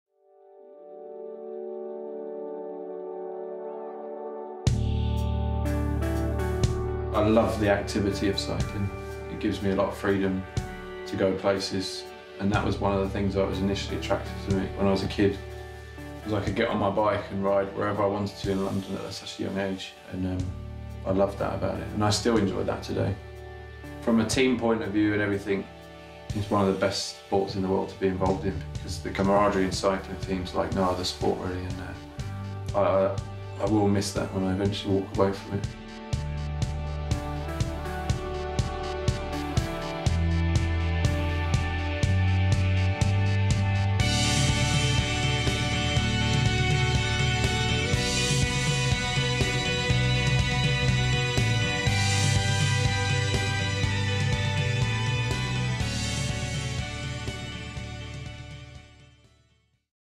Wiggins - Why I Love Cycling
Why do you love riding your bike? Our friends at GoSkyride asked Sir Bradley Wiggins just that, and this was his reply.